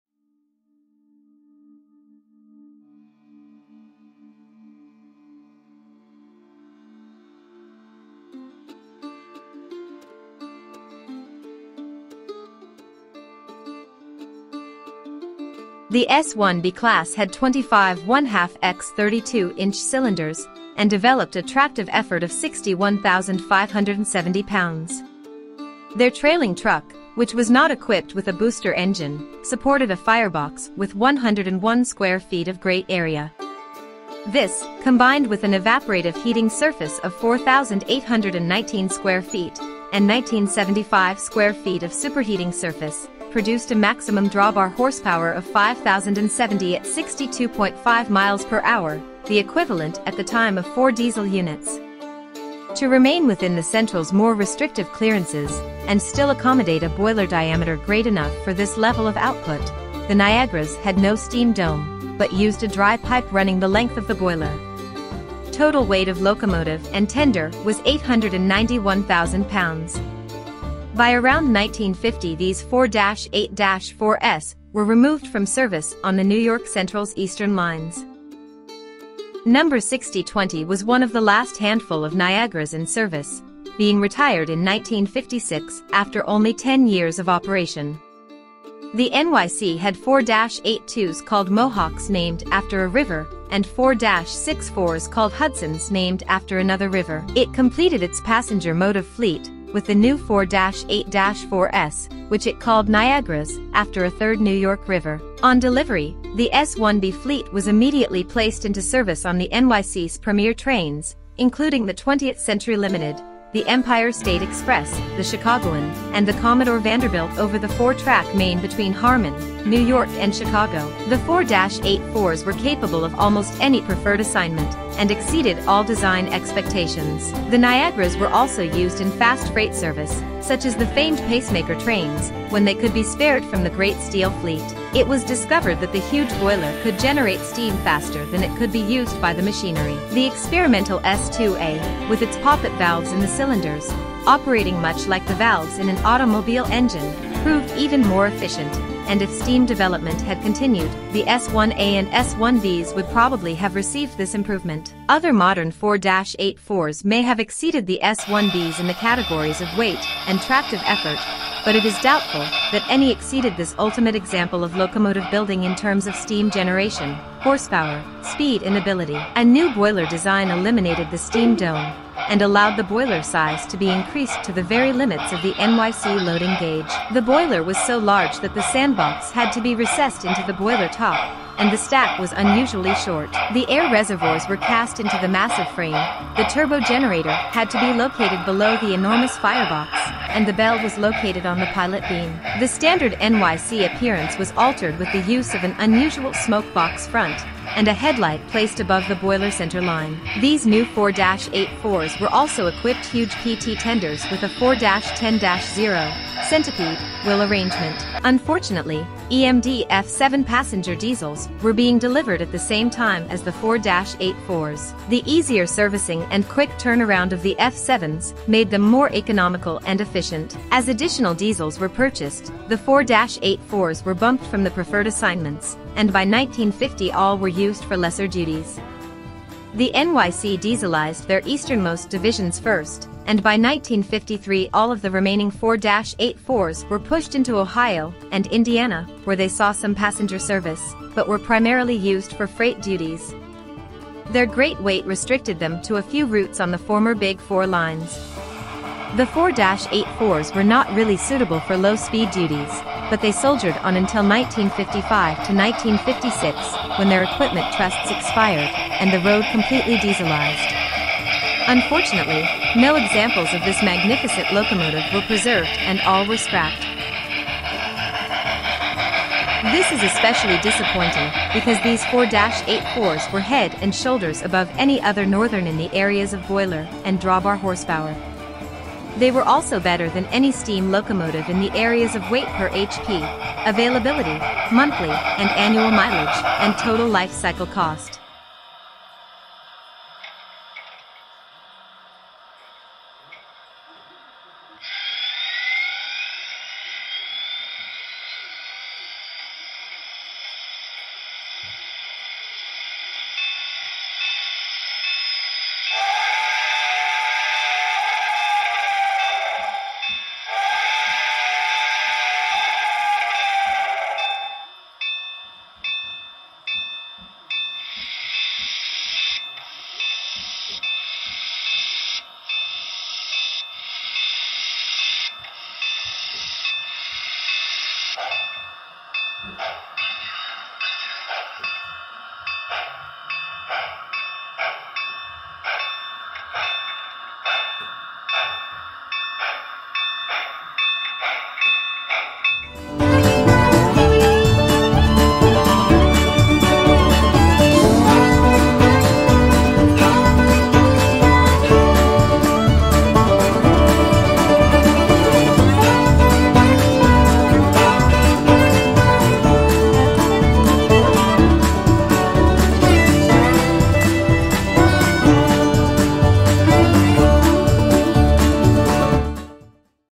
Broadway Limited NYC Niagara S1b 4-8-4, #6020
Broadway Limited HO Scale NYC Niagara S1b 4-8-4, #6020, Paragon3 Sound/DC/DCC, Smoke, HO

The New York Central was late in adopting the 4-8-4 wheel arrangement for their steam locomotive roster since the Mohawks and Hudsons did such an excellent job. Locomotive #6000-- class S1a -- was the first 2-cylinder 4-8-4 from ALCO and was followed by 25 more of the S1b class. When the "Northerns" started arriving in 1945, they became the favorite power, being used almost everywhere at once. They were dual service locomotives, but are mostly remembered for heading up the "Limiteds". Now, for the first time in Paragon3, with Rolling Thunder TM, the New York Central Niagara will ride again in HO scale!

FEATURES

- NEW Paragon3 Sound & Operation System FEATURING ROLLING THUNDERTM with Authentic Sounds and Prototypical Operation in both DC and DCC environments
- Synchronized Puffing Smoke with each Chuff
- Variable Puffing Smoke Intensity and Timing
- Locomotive Composition: ABS Body with Die Cast Chassis
- Tender Composition: ABS Body with Die Cast Chassis
- Couplers: (2) Operating Kadee or Compatible
- Compatible Tracks: Code 70, 83, 100 Rail
- Equipped with Traction Tires
- Minimum Operating Radius: 22 in or greater